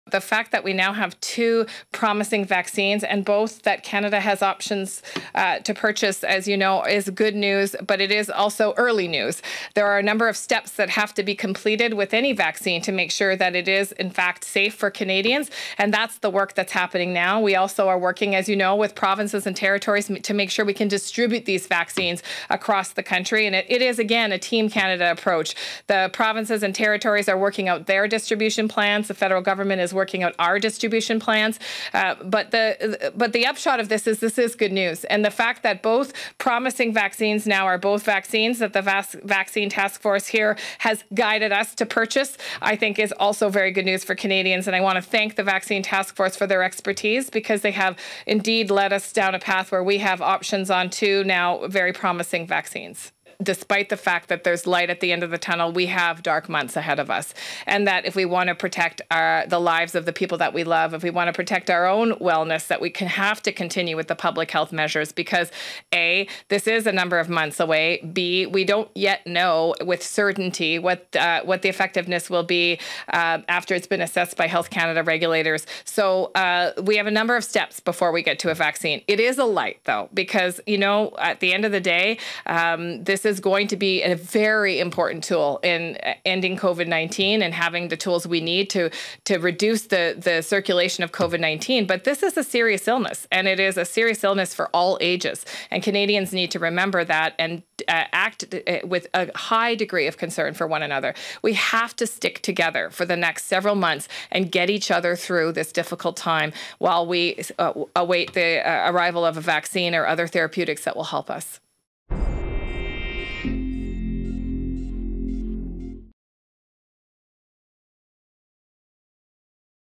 National plans for COVID-19 vaccine distribution in the works: Hajdu
Canada has options for millions of doses of promising vaccines against COVID-19, Health Minister Patty Hajdu says, but getting them distributed across the country will be a big job. She says there are lights at the end of the tunnel but it's critical not to let the virus spread before vaccines are available in several months.